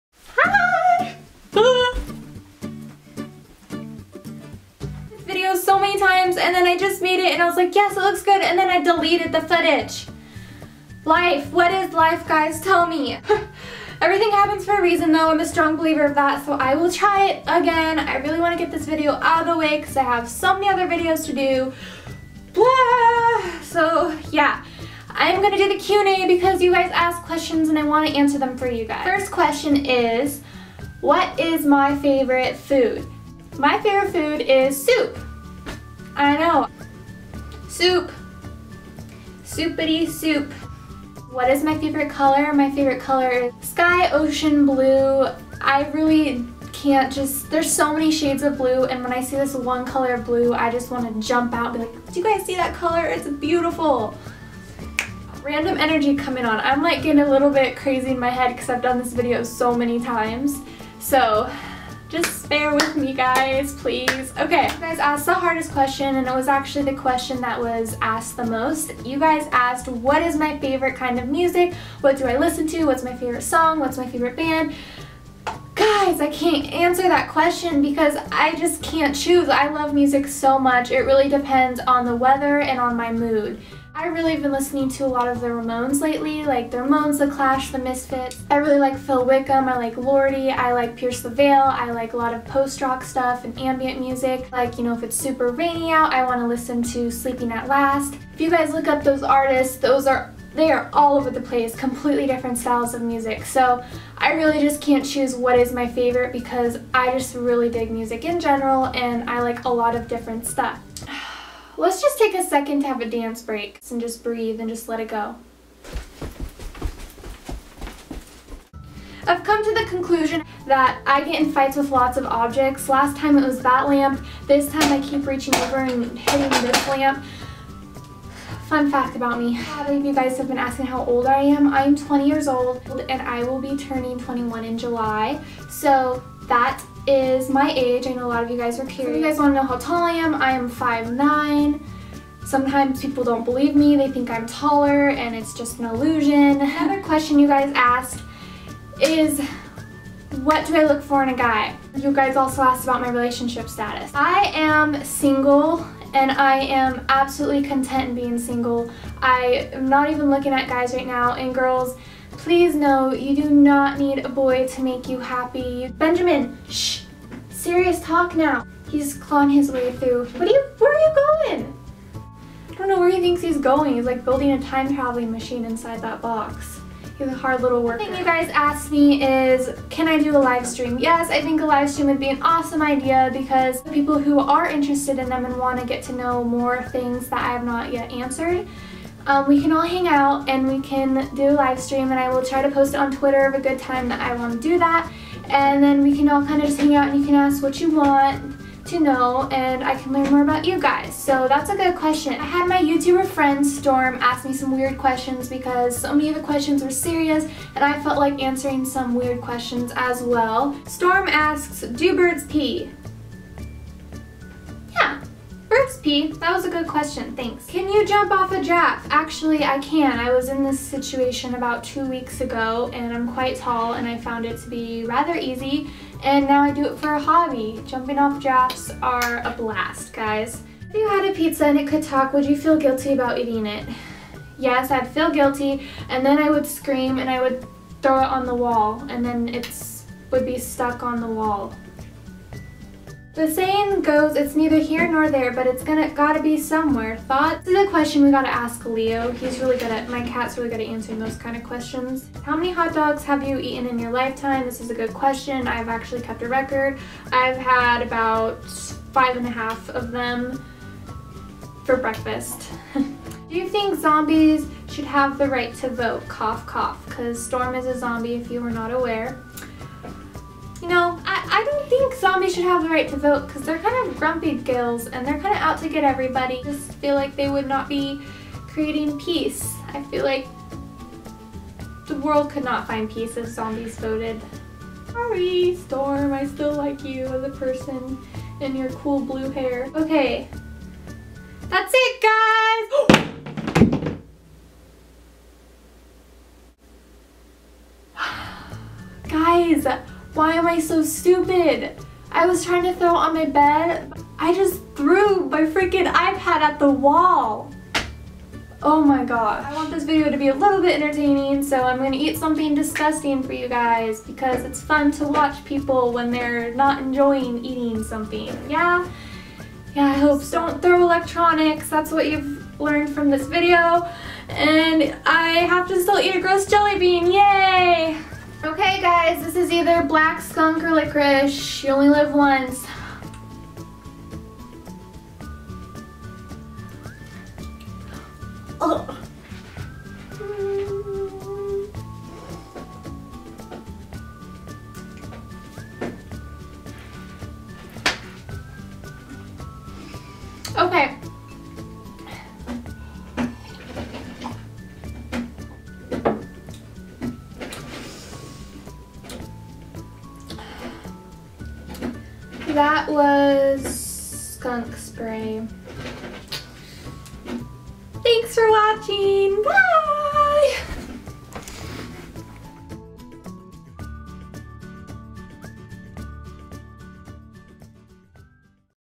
Q&A | RachelHaylie
Today I answer some of the questions you guys asked! Listen closely and you can hear my rabbit in the background working on a teleportation device, so that's cool. I also eat a jelly bean that is either licorice or skunk spray and finally, I hope you guys enjoy all the arm flailing that is involved in this video!

Come say hi to me on Twitter: Twitter.com/RachelHaylie_